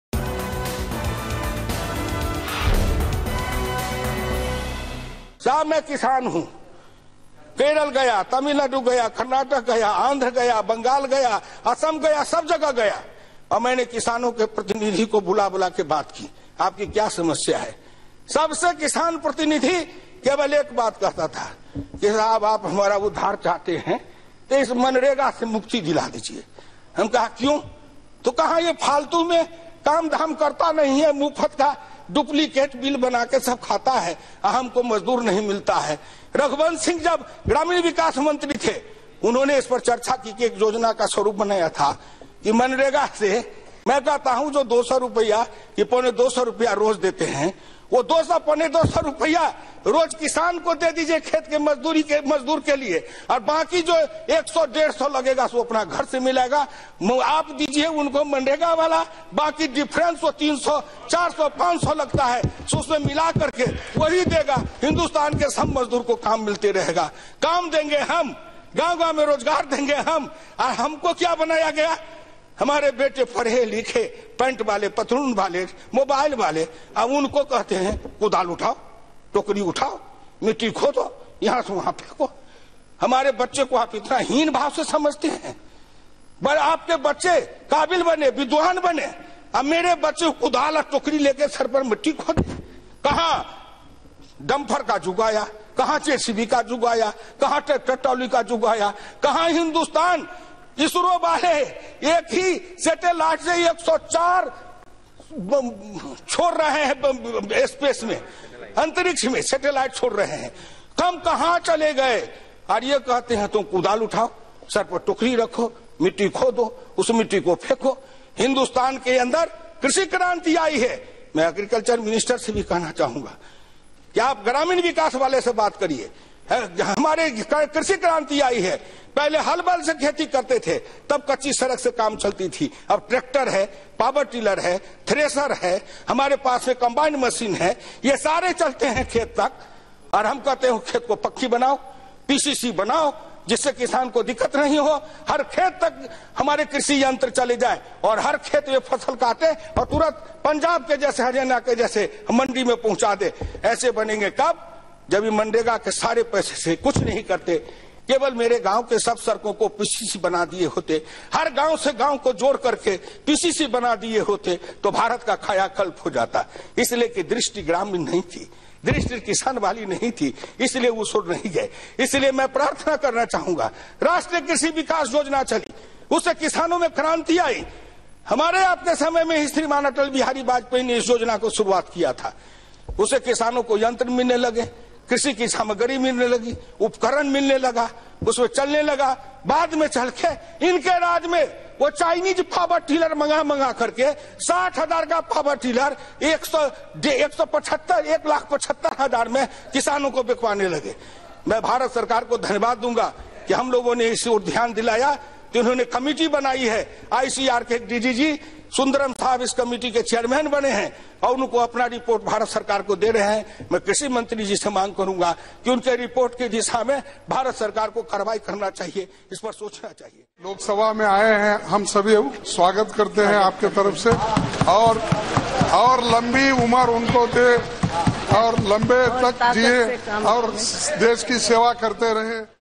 Hukmdev Narayan Yadav Emotional Speech Over Poor Staying Poor | Fraud in Wages to Daily Workers
Hukmdev Narayan Yadav emotionally gave a speech on the daily wage workers facing problems as the dealers who are in between are taking away the money by producing fake bills as the poor is still staying poor as the rich is becoming richest.